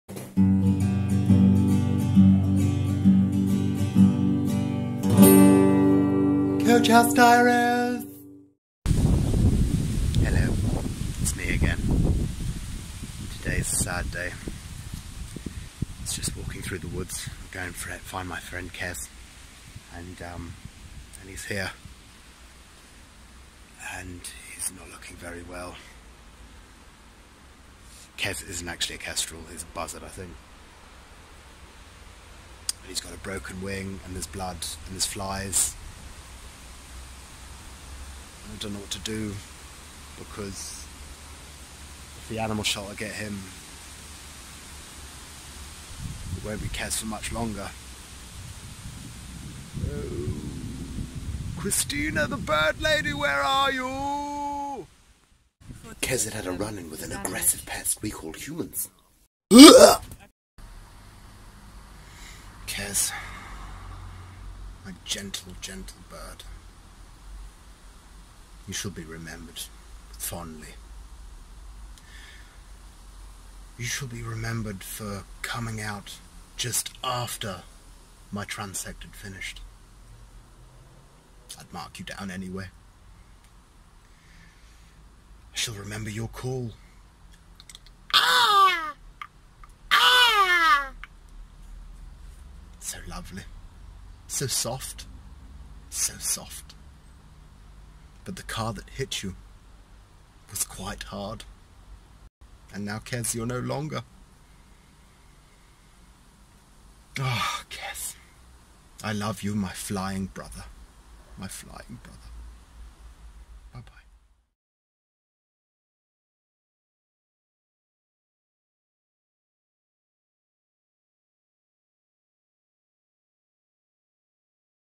The coach house diaries E6. Sad times in St Peters Valley
As Rufus visits his friend, he is confronted with a sad dilemma.